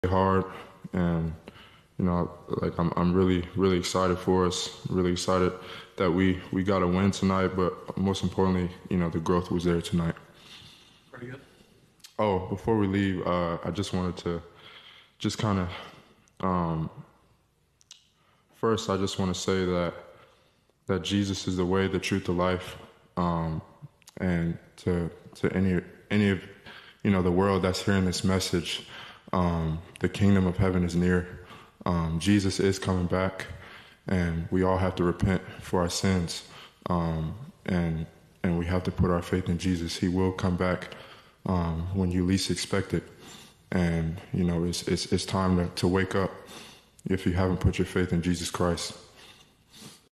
"Jesus is Coming Back" Jaden Ivey assured during the postgame interview
Detroit Pistons guard Jaden Ivey speaks to media, after the win against the Brooklyn Nets during Pistons postgame interview earlier this month.